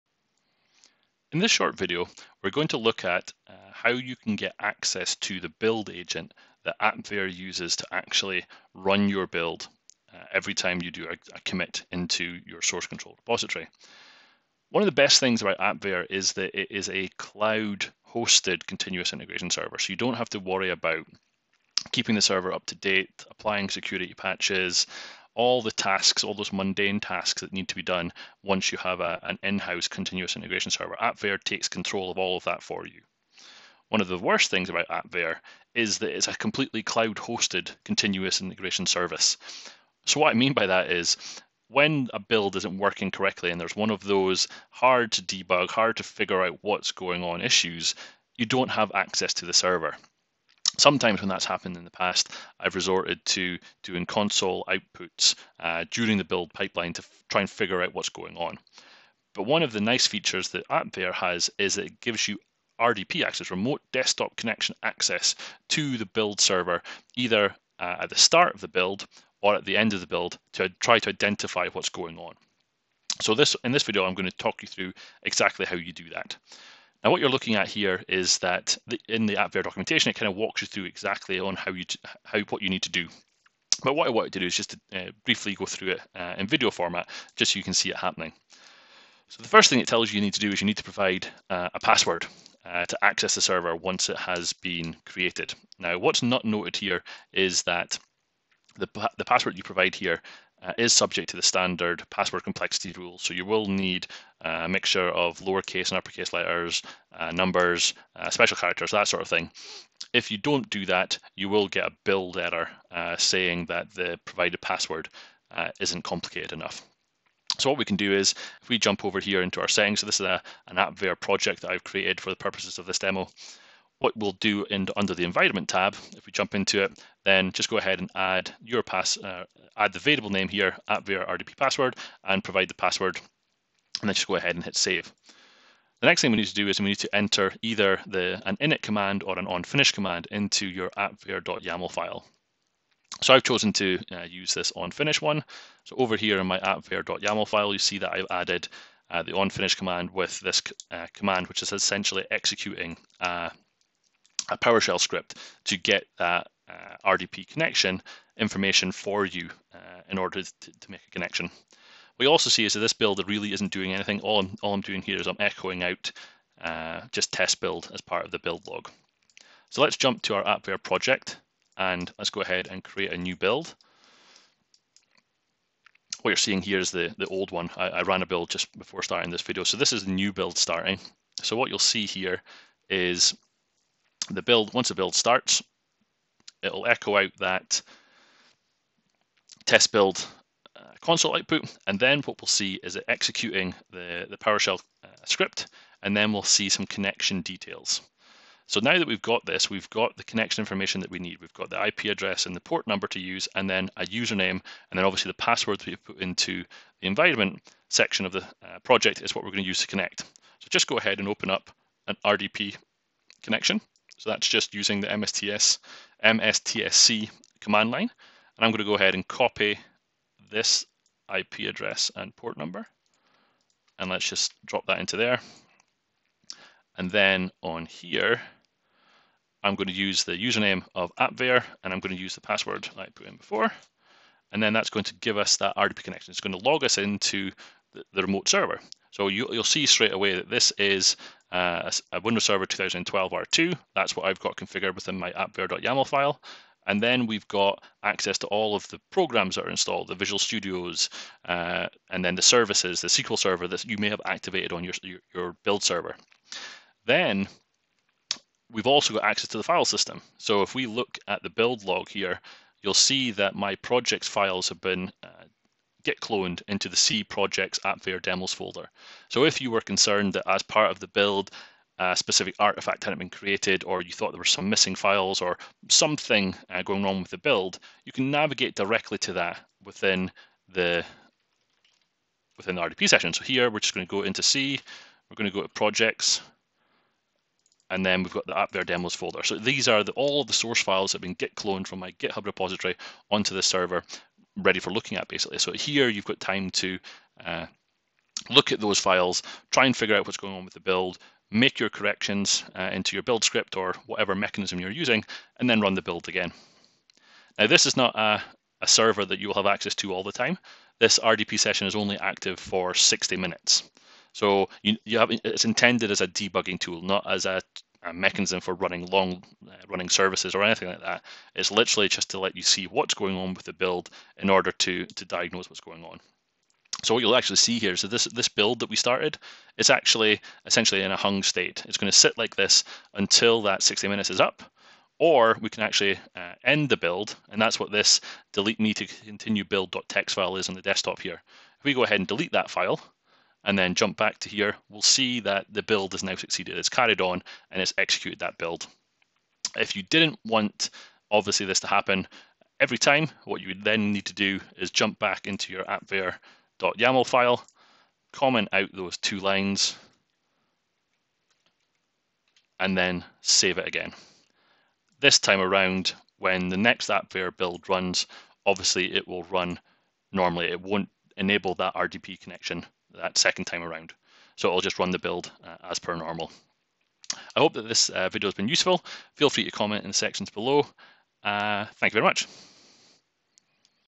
How to use AppVeyor Remote Desktop Connection
This video introduces the ability to establish a remote desktop connection to the AppVeyor Build Agent, in order to allow debugging of troublesome builds.